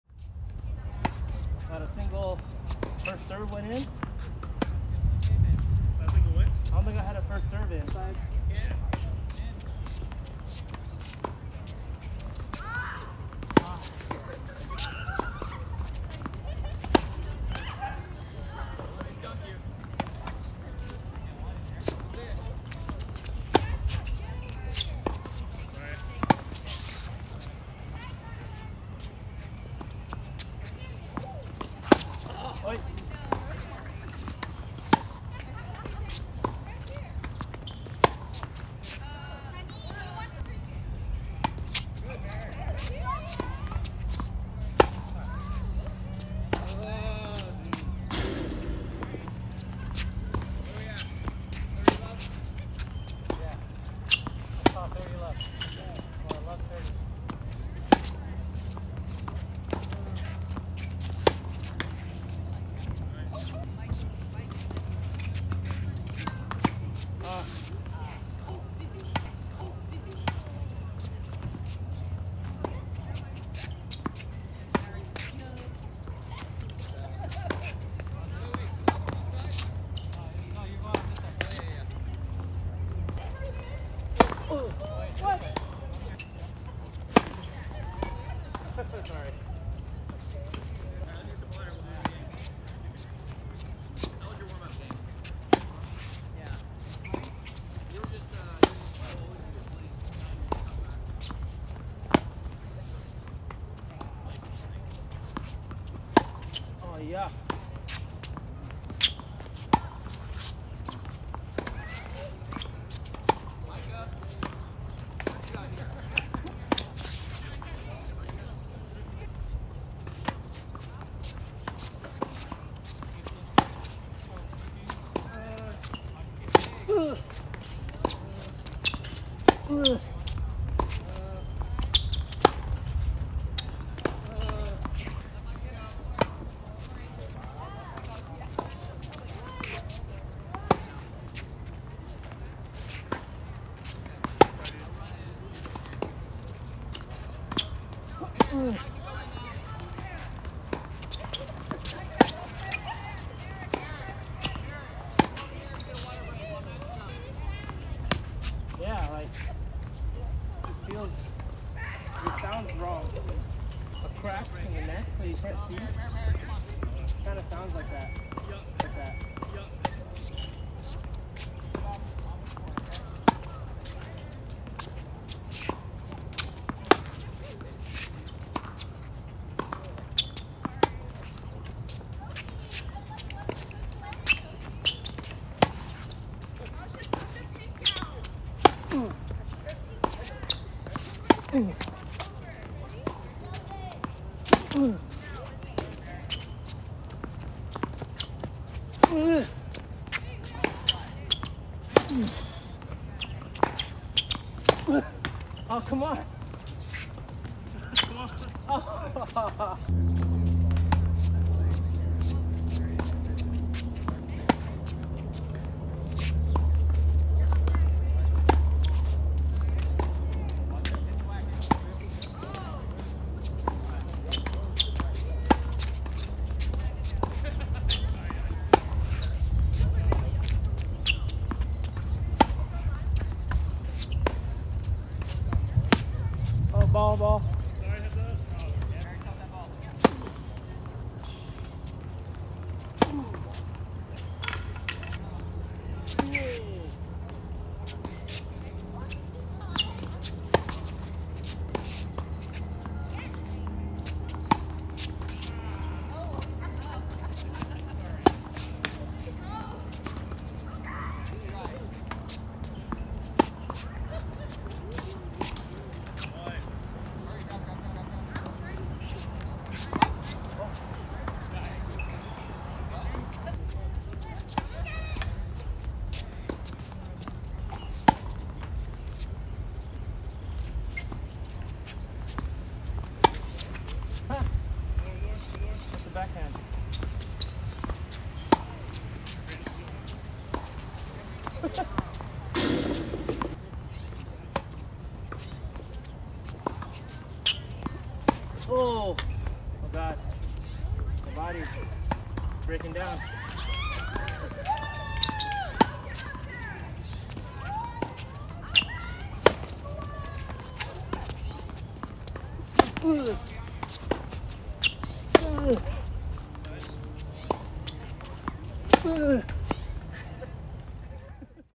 Extra BSOTM: Post Match Rally w/ VCORE 95
I normally hit with a VCORE PRO 97, as seen in other videos.